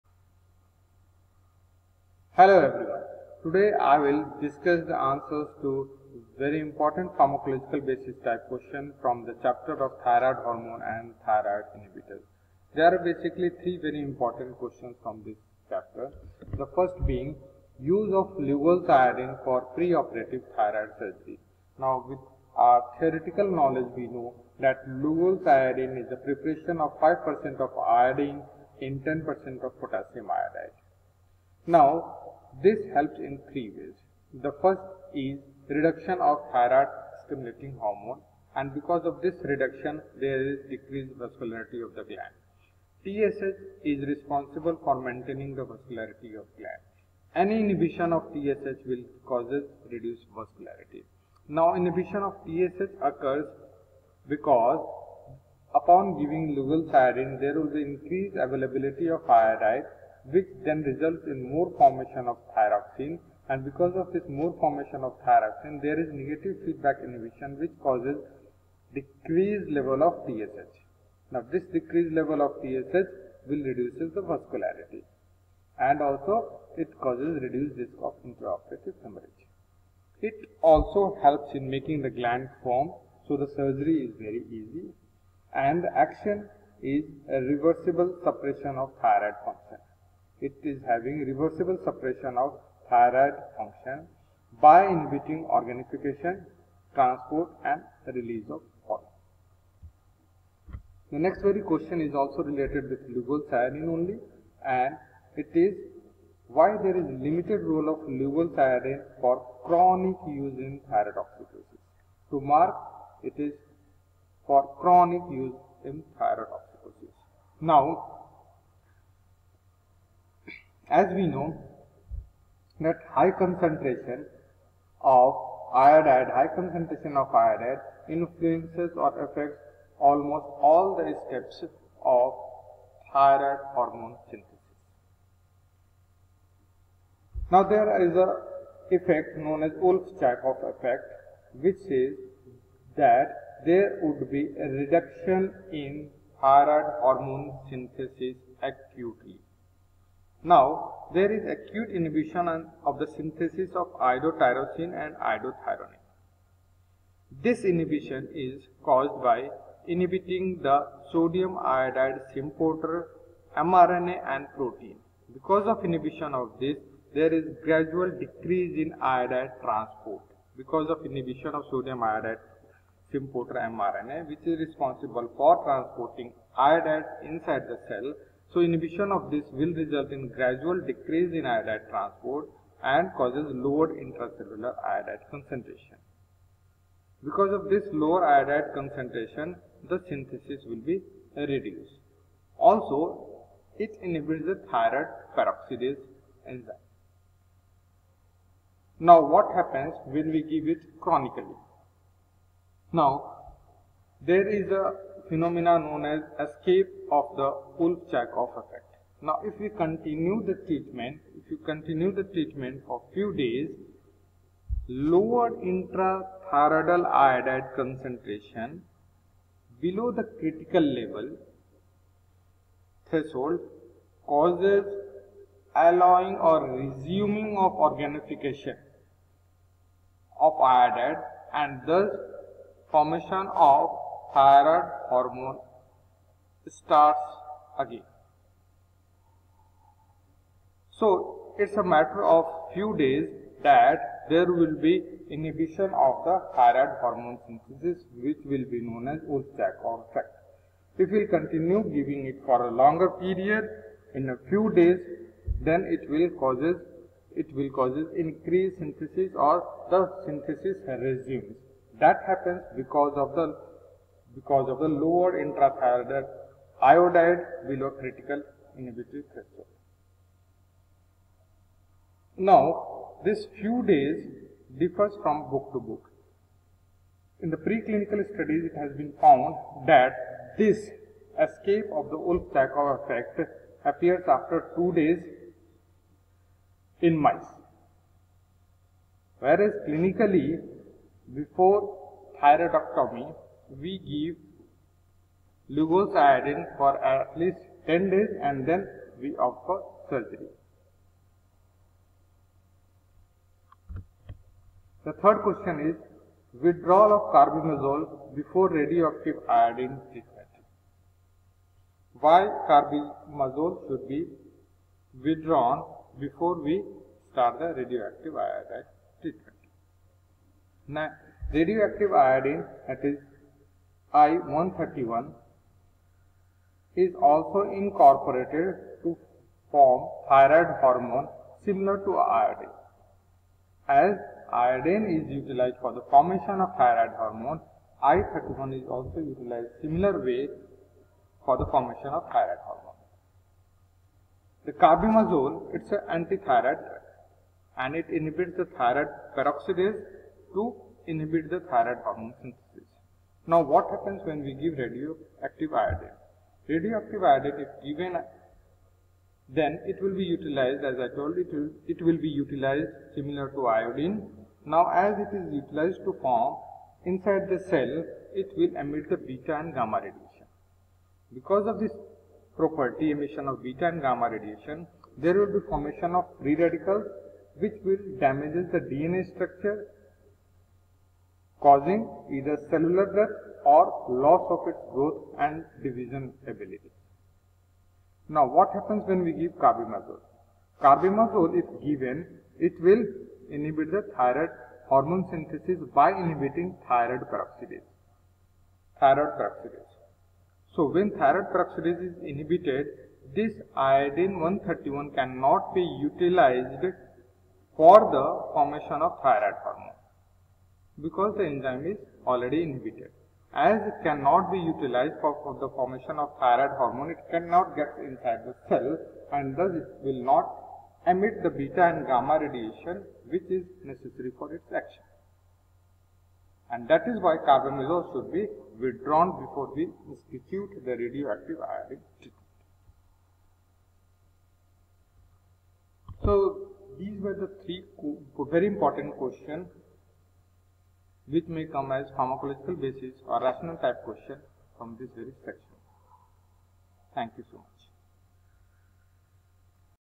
Thyrotoxicosis, wolf chaikoff's effect and more
This video describes answers to rationale type question of thyroid hormone and anti-thyroid drug chapter.
-3 questions are explained in here.
Q1. Use of Lugol’s iodine for preoperative thyroid surgery.
Q2. Limited role of lugol’s iodine for chronic use in thyrotoxicosis.
Q3. Withdrawal of Carbimazole before radioactive iodine treatment.